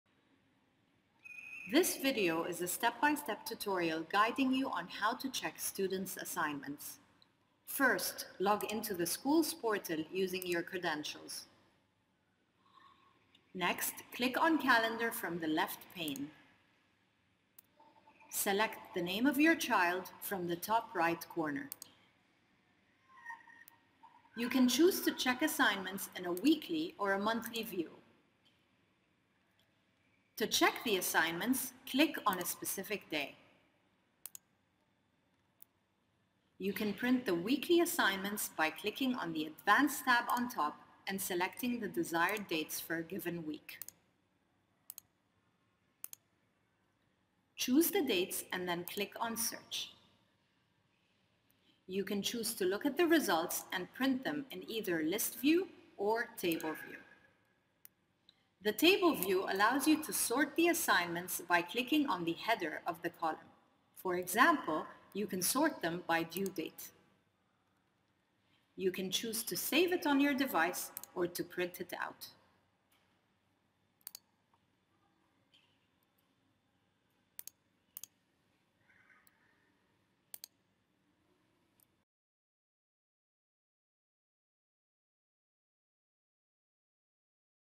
Al Mawakeb School Calendar
This video shows how parents can view the School Calendar from the school portal.